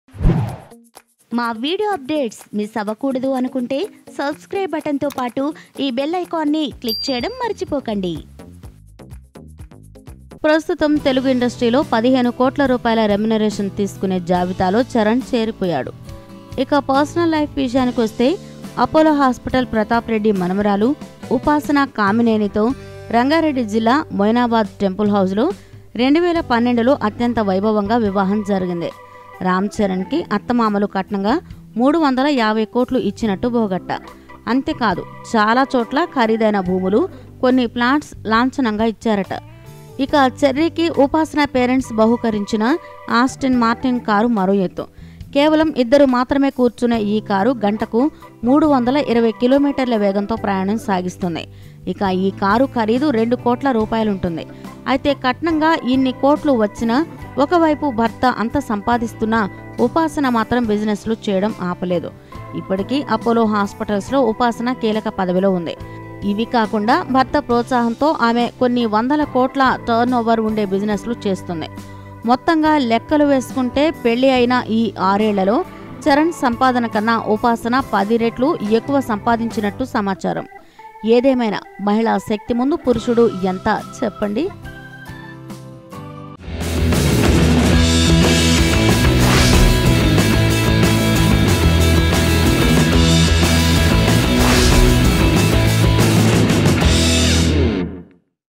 Upasana's Revenue Income After Marriage | Kamineni Upasana Businesses & Income Profit | Telugu Panda
Upasana's Revenue Income After Marriage. Kamineni Upasana Businesses and Income Profits are higher than Ram Charan Tej.